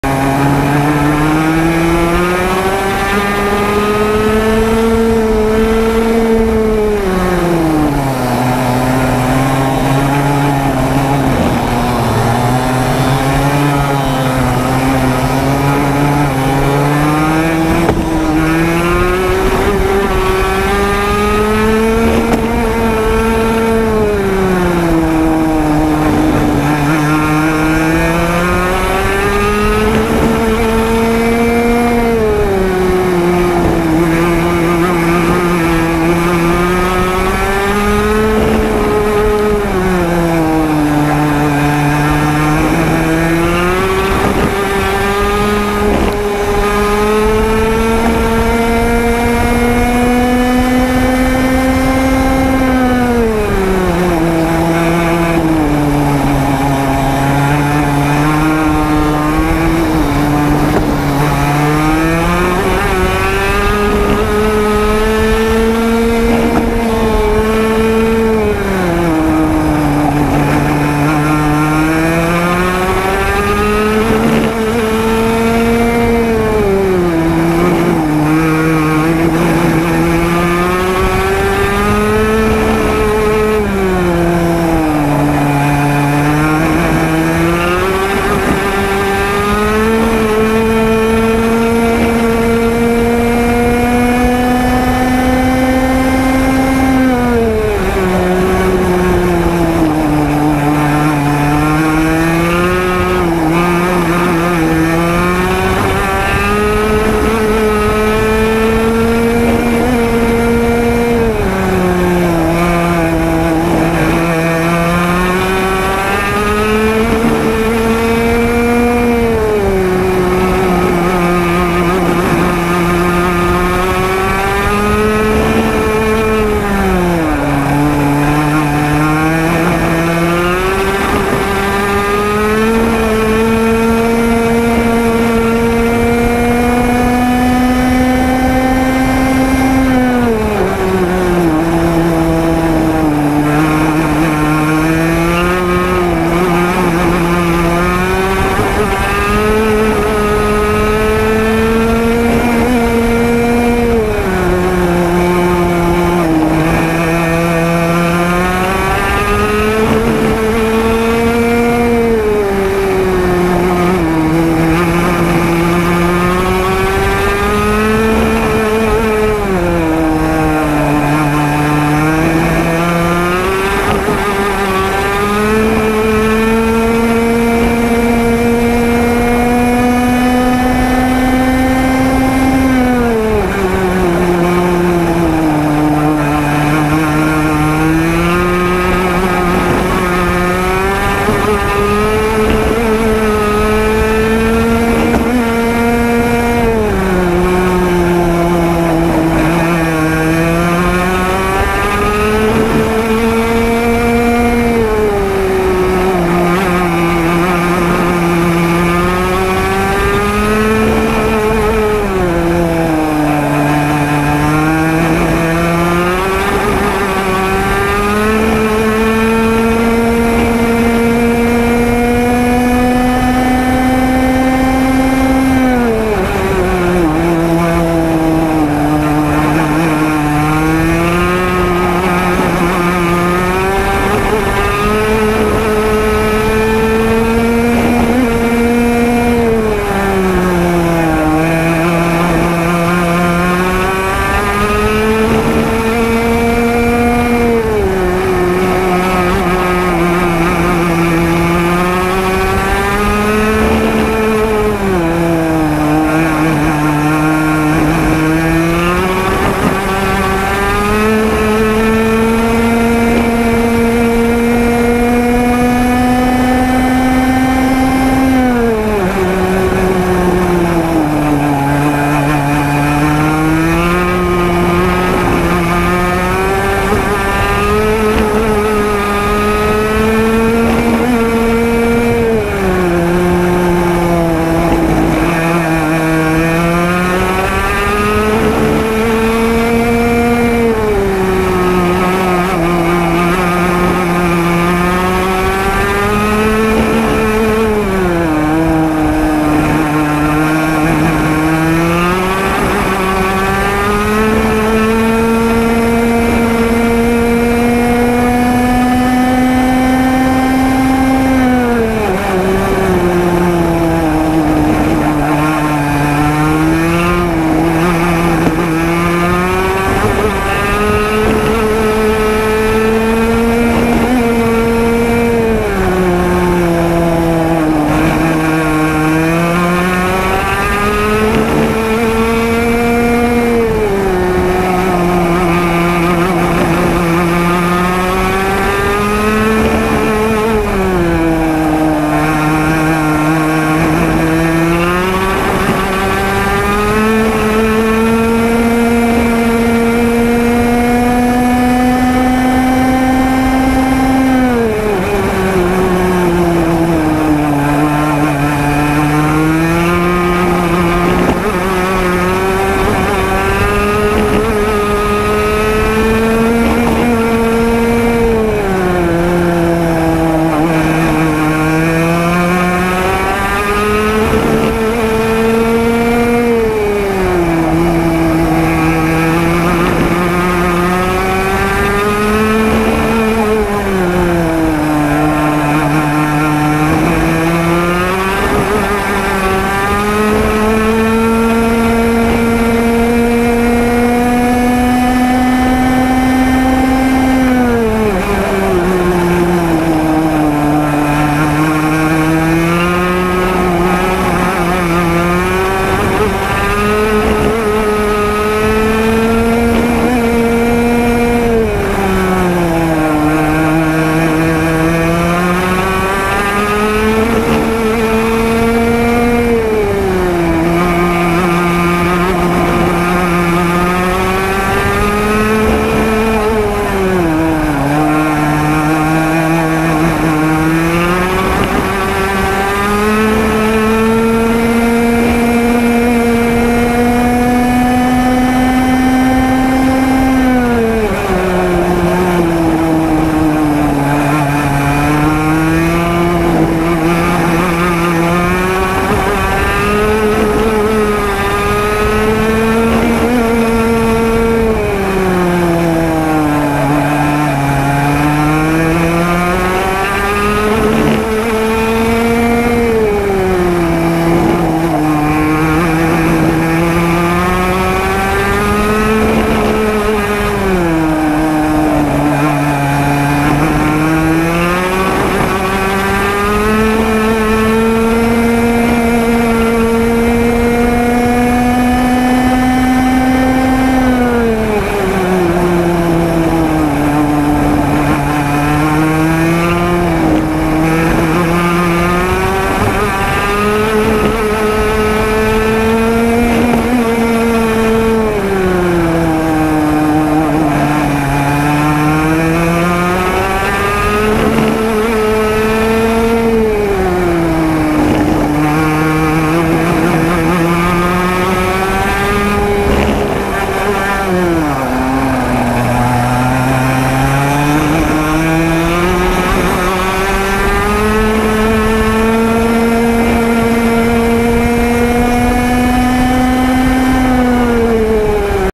Heat 1, Xtreme speedway kart race China Round 2, 2015年極速杯小型賽車大賽第二回合
Open 125 class 排位賽 1/Heat 1 (12 laps)
Gopro HD Hero 4 black edition, 1080P30FPS
Explorer chassis with IAME X30 125cc engine